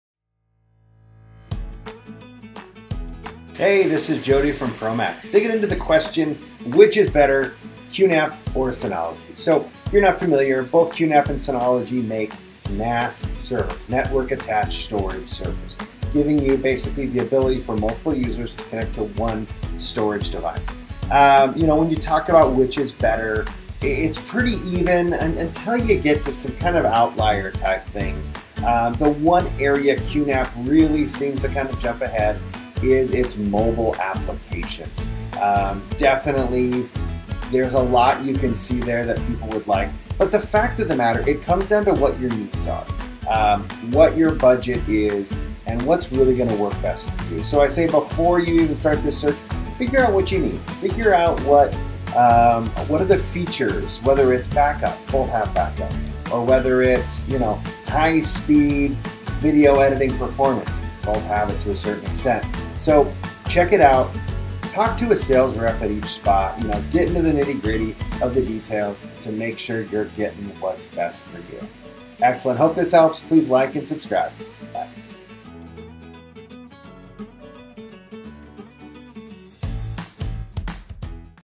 QNAP vs Synology: Which is Better? NAS Shared Storage for Video Editing
This is a new series comparing NAS devices QNAP and Synology. You can read more about this question and get into the bigger topic below. NAS Shared Storage for Video Editing